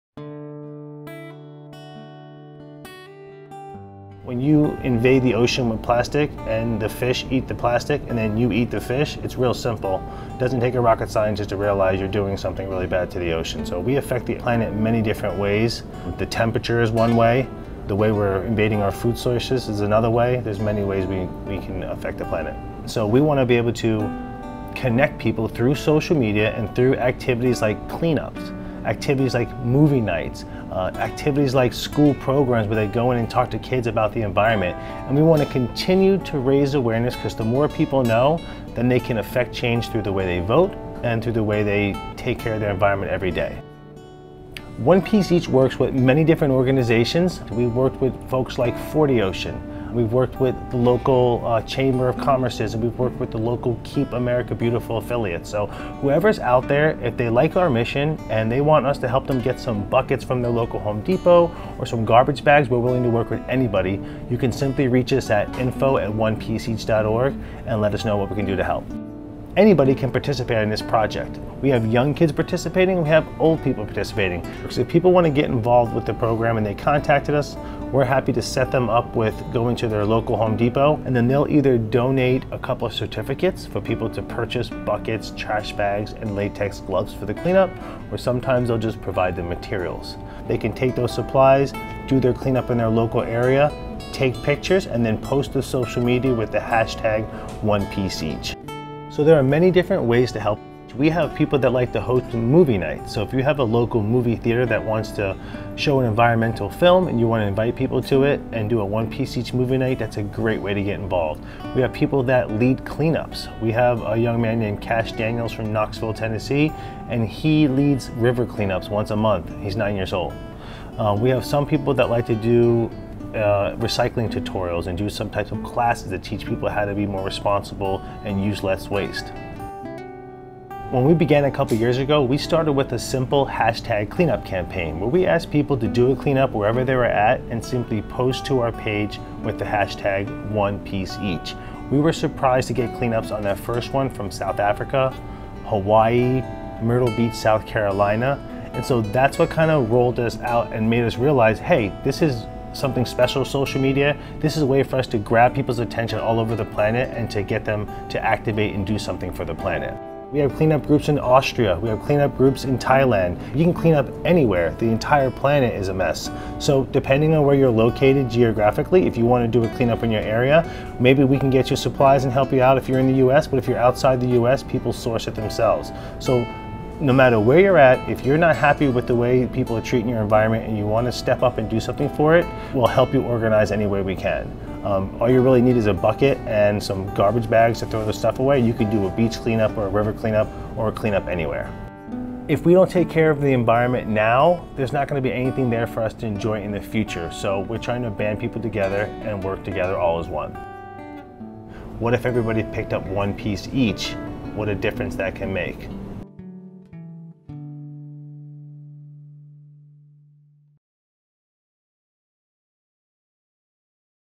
1 Piece Each
I created a video to show the great work 1 Piece Each is making for our environment and for our community. It has been an honor to be part of this mission and I hope my work and the video help to spread the need we have to take care of our planet.